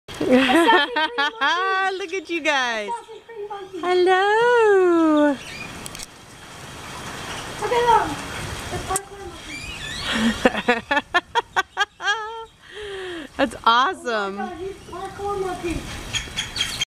Monkeys at the Natural Bridge Wildlife Park
March 2018, San Antonio, Texas, Natural Bridge Wildlife Park
After you are done driving through the park, there is a whole separate area with a petting zoo, and cages with monkeys, and lemurs!  It was like a whole bonus section, we spent just as much time there as we did driving through the park, it was awesome! Ryder thought they were doing "park core" workouts :)